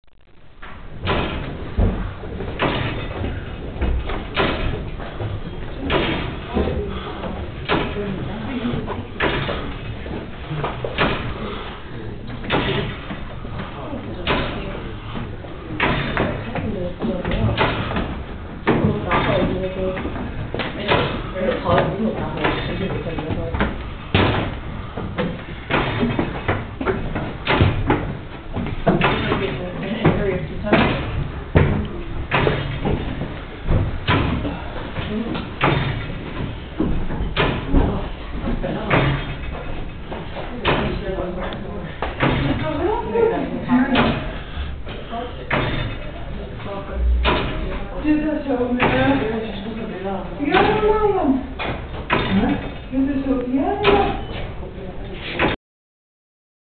Belfry mechanism in Bruges, Belgium
This is what's at the top of the belfry in Bruges.  You don't see this in the movie "In Bruges".